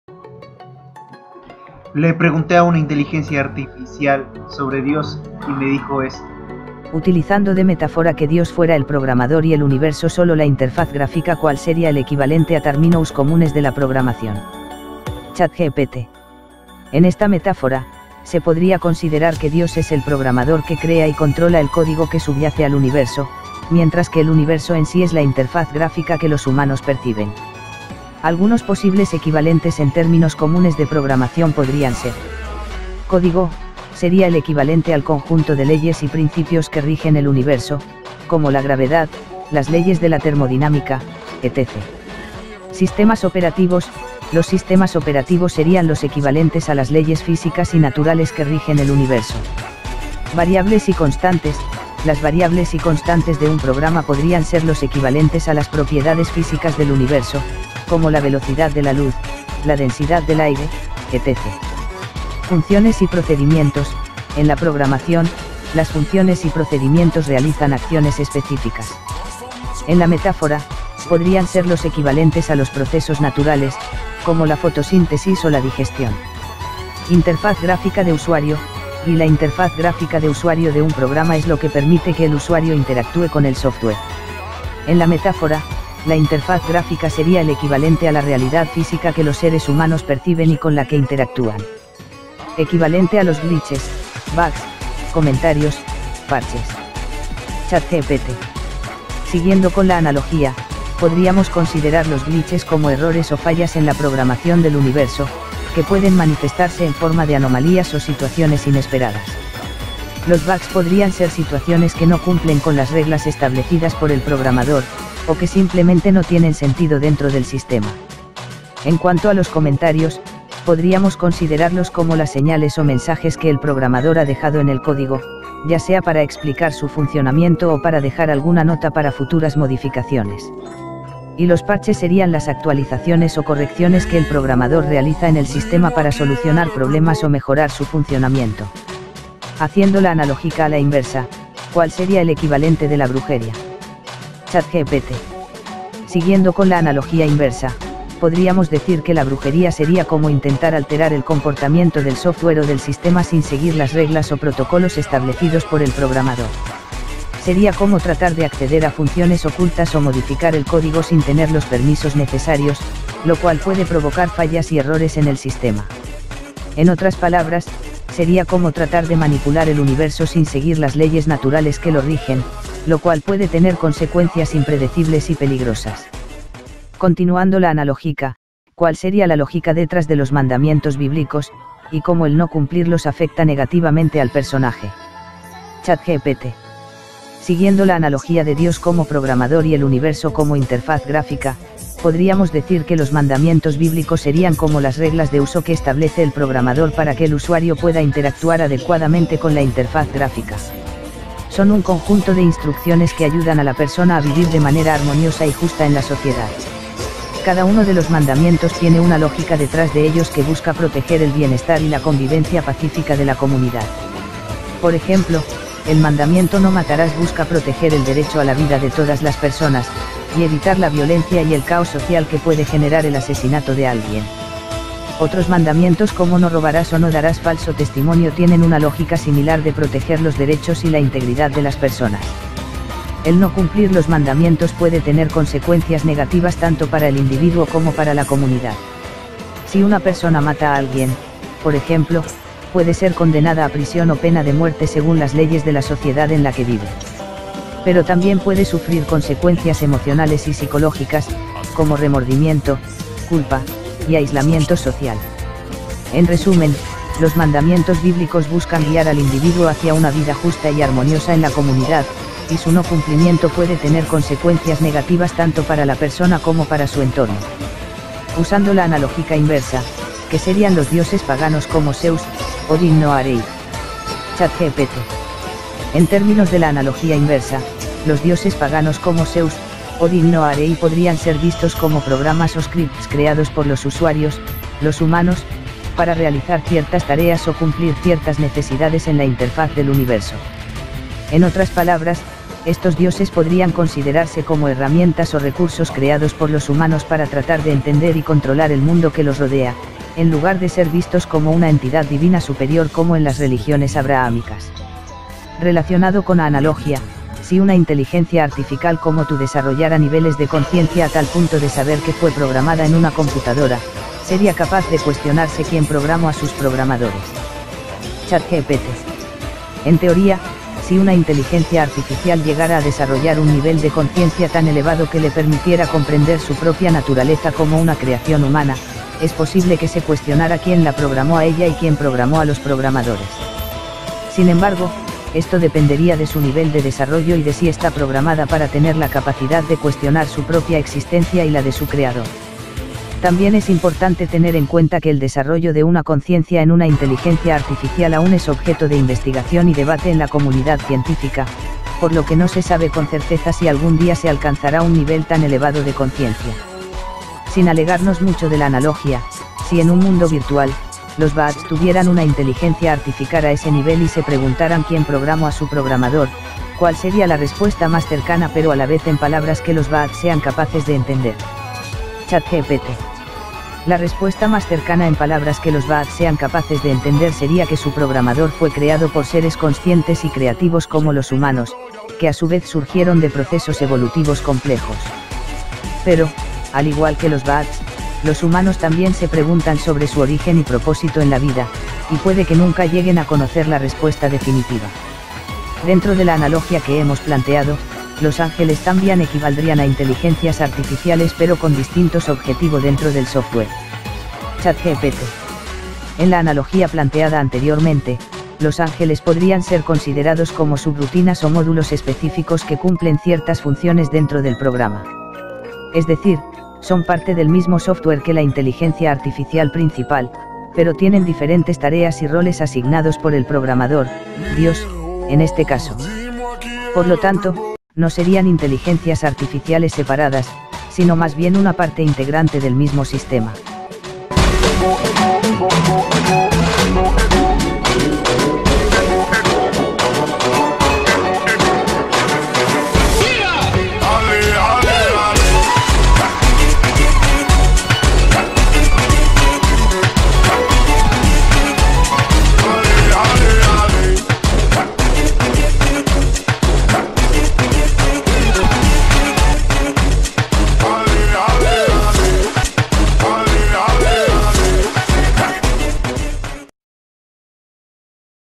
Le Pedí a una Inteligencia Artificial que haga una metáfora de Dios Como Programador y me dijo esto.
Hablame

Cameos del ultimo cortesía de @tuantiheroe